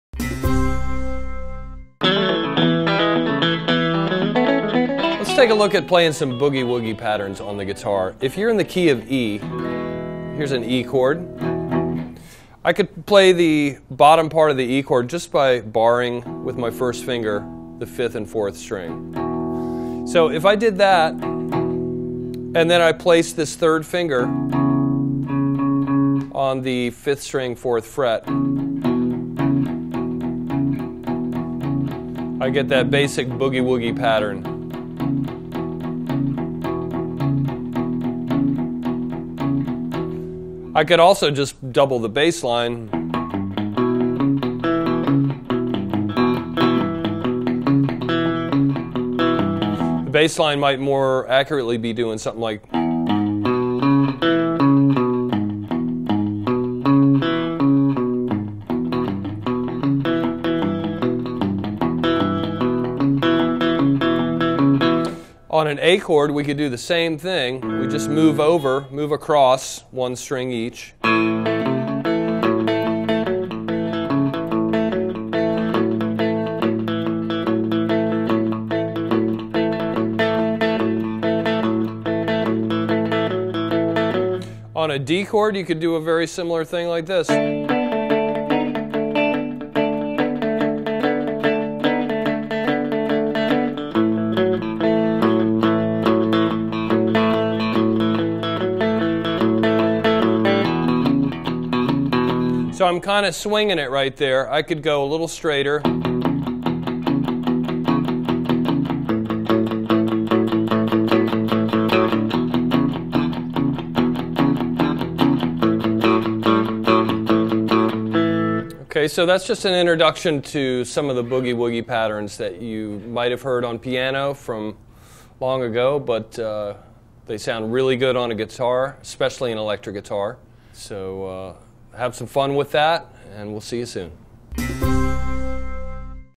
How to Play Boogie Rhythm Patterns | Country Guitar
Let's take a look at playing some boogie-woogie patterns on the guitar. If you're in the key of E, here's an E chord. I could play the bottom part of the E chord just by barring with my first finger the fifth and fourth string. So if I did that and then I placed this third finger on the fifth string fourth fret, I get that basic boogie-woogie pattern. I could also just double the bass line. The bass line might more accurately be doing something like.

On an A chord we could do the same thing. We just move over, move across one string each. On a D chord you could do a very similar thing like this. So I'm kind of swinging it right there. I could go a little straighter. Okay?

So that's just an introduction to some of the boogie-woogie patterns that you might have heard on piano from long ago but they sound really good on a guitar, especially an electric guitar. So have some fun with that and we'll see you soon.